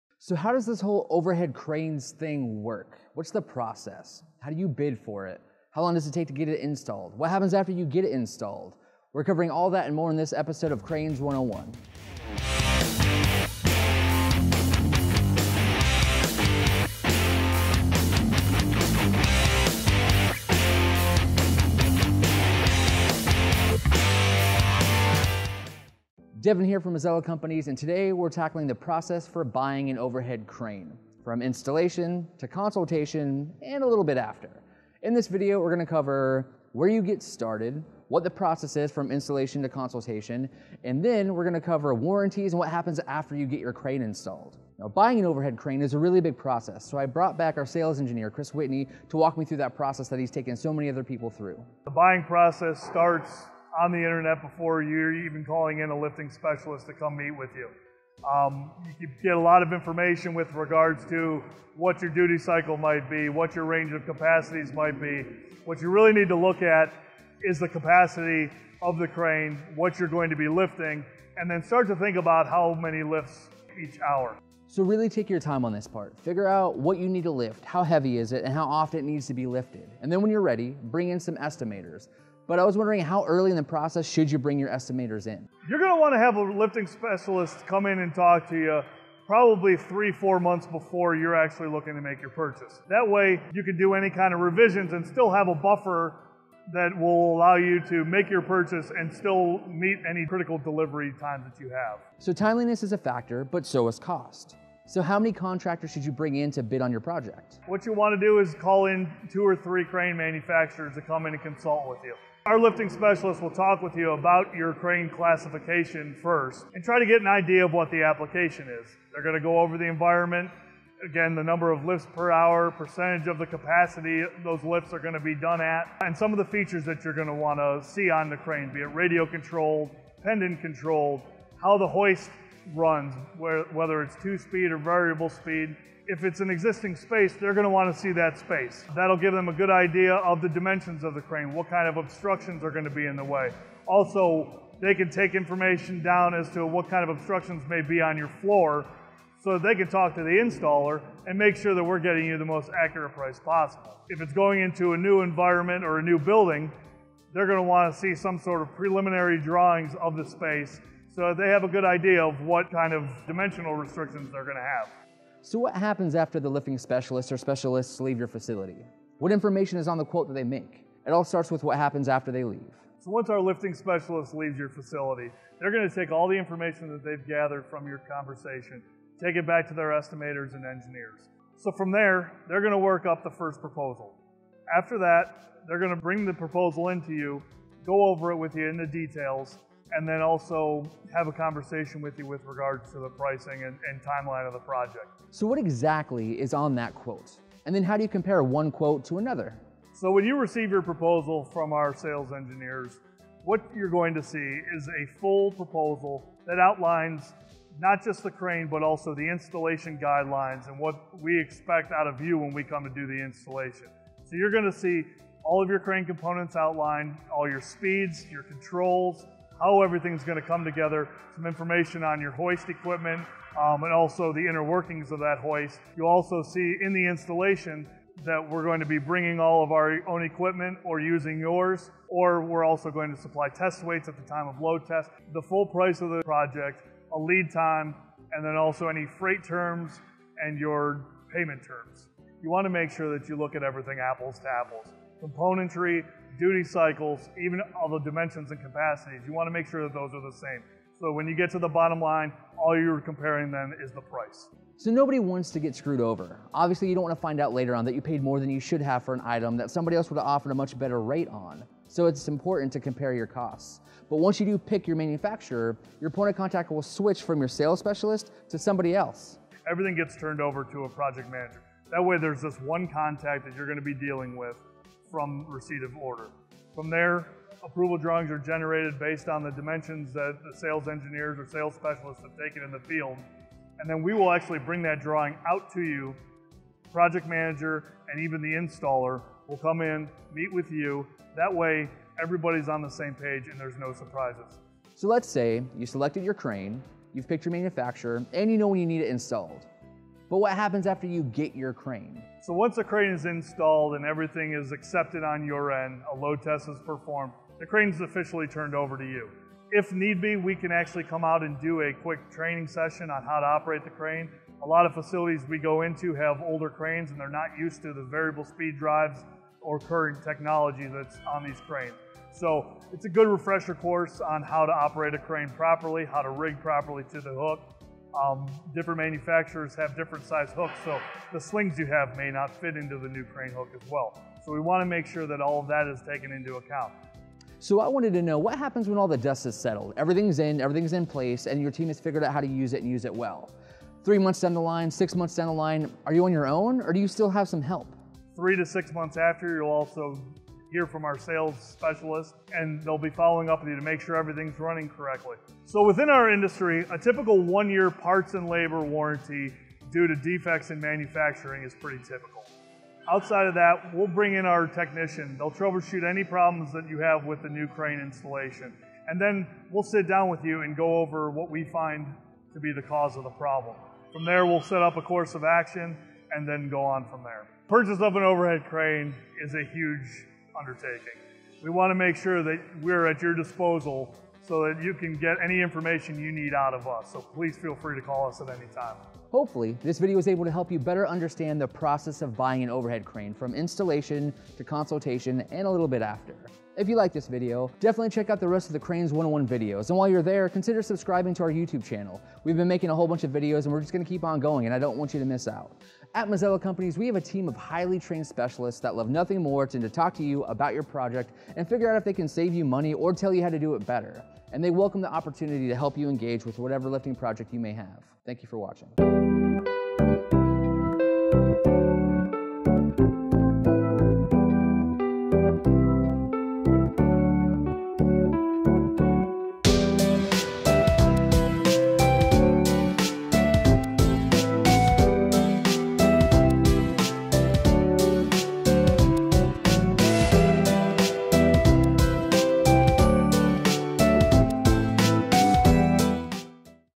Buying an Overhead Crane: Consultation to Installation & Beyond
There are a lot of steps between buying an Overhead Crane and getting your overhead crane installed. In this Episode of Cranes 101, we aim to explain the entire process from Consultation to Installation, and beyond!
In this video we cover:
0:00 - Intro
0:37 - What we'll cover
0:56 - What you do before reaching out to a Lifting Specialist
1:25 - When should you bring in an Estimator?
1:49 - How many contractors should you call?
2:55 - What happens after the initial crane consultation?
4:33 - Mazzella's Project Management process
5:01 - What happens after the Crane is Installed?
5:50 - Mazzella's follow up
6:00 - Overhead Crane inspection and warranties
6:39 - Closing